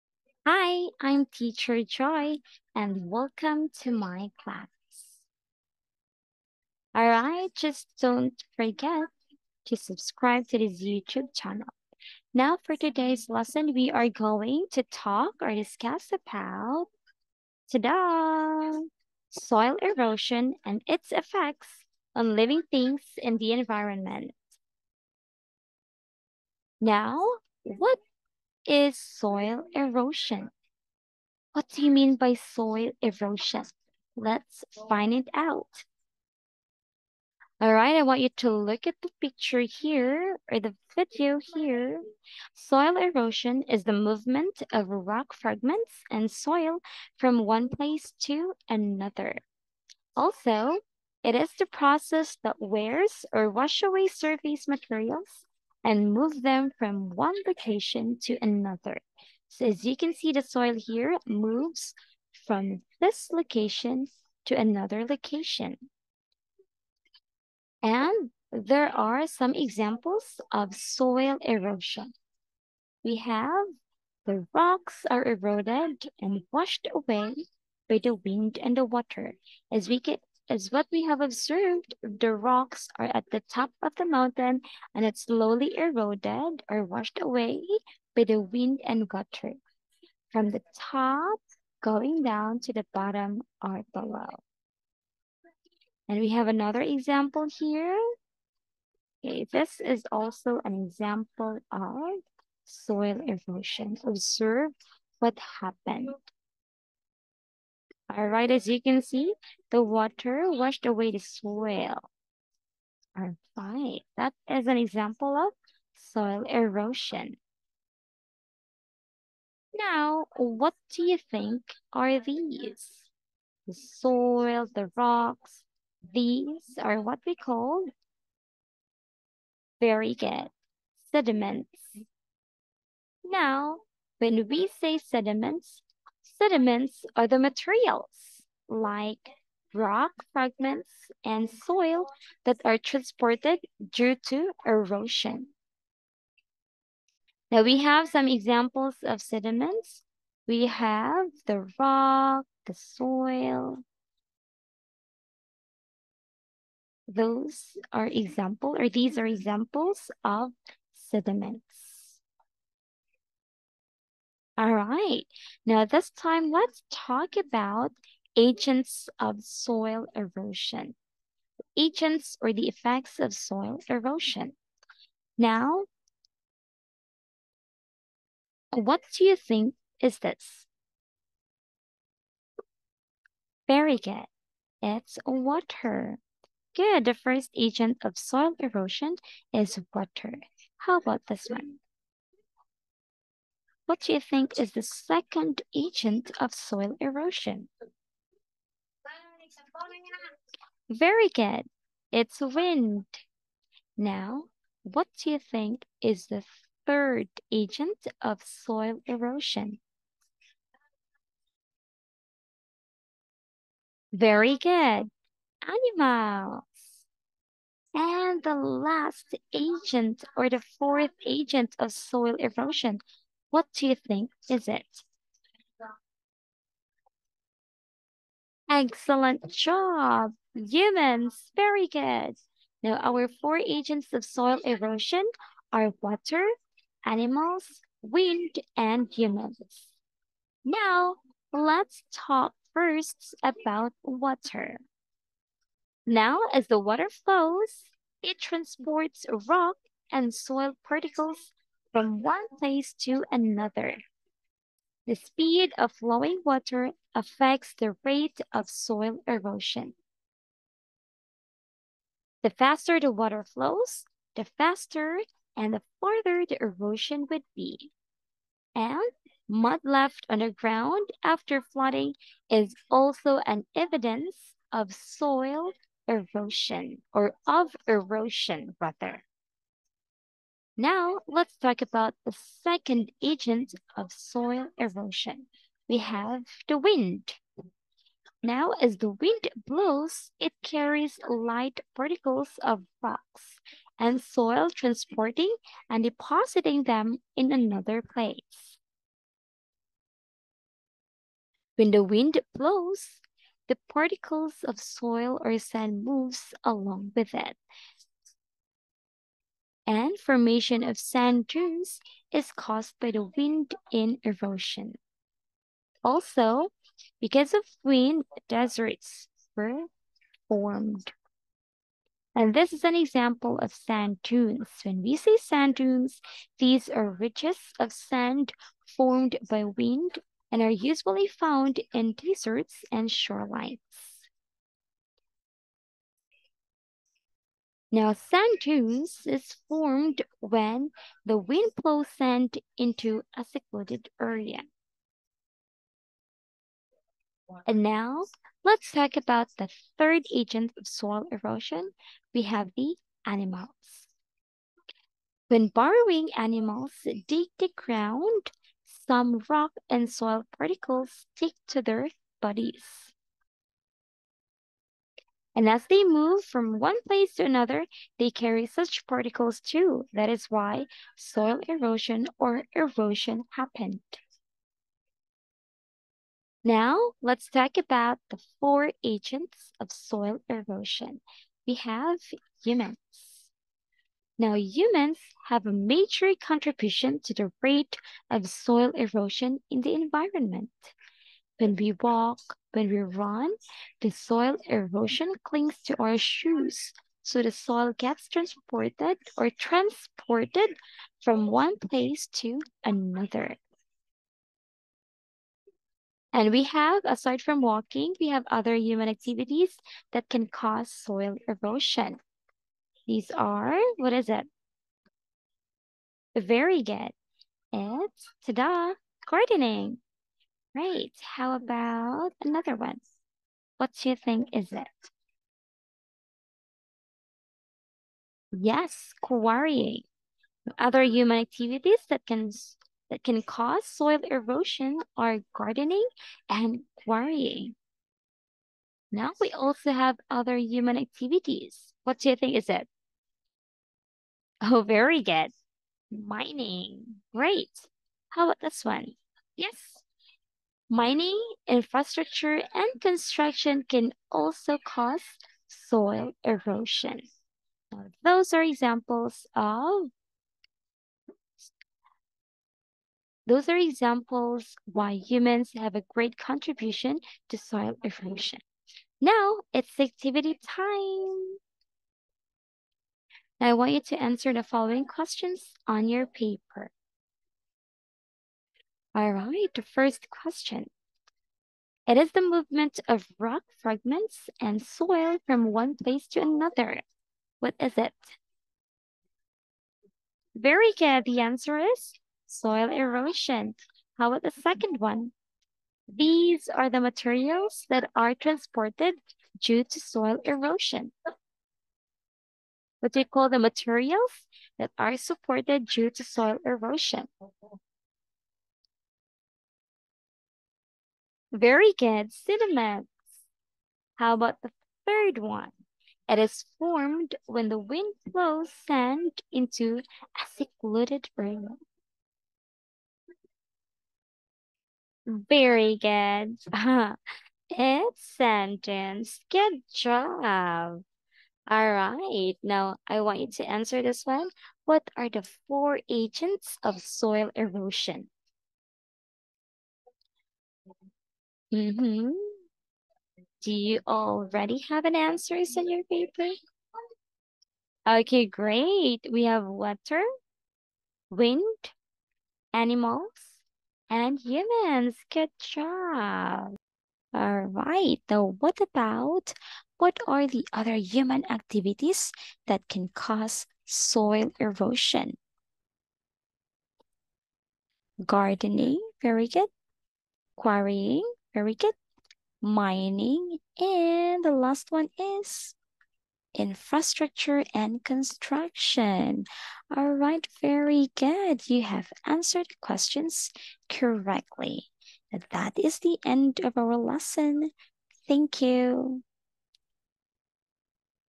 Soil Erosion and its Effects on Living Things and the Environment | Grade 5 | by: Teacher Joy
Hi! I'm Teacher Joy and welcome to my class. 😄

For today's lesson we are going to talk about "Soil Erosion and its Effects on Living Things and the Environment".

Happy learning! 😊